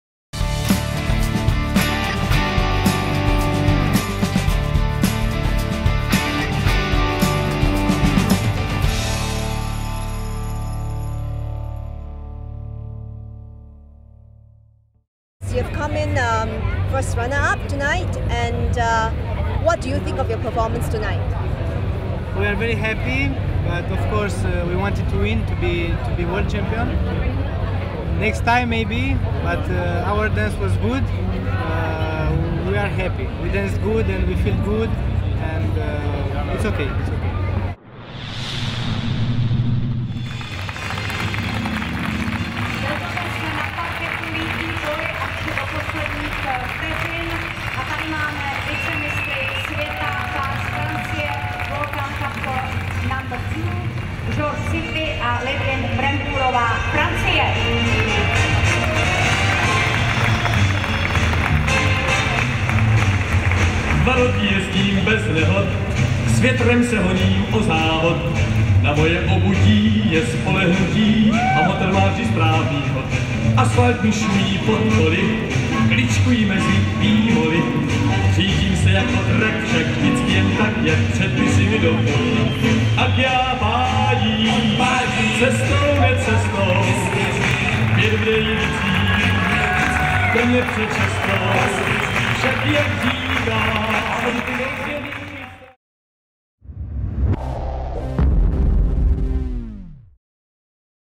2011 World Youth Latin Gala: Sutu - Brangbour, FRA
Czech public broadcaster CESKA TELEVIZE had the six finalist couples of the World DanceSport Championship Youth Latin Championship feature prominently in a 90-minute Gala programme. It aired live during primetime hours on 1 October. Sutu George - Brangbour Ludivine, FRA